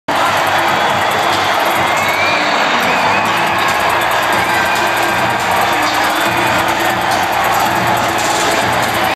Leafs Party Celebrates Goal
The ACC after the Leafs tie it up 2-2.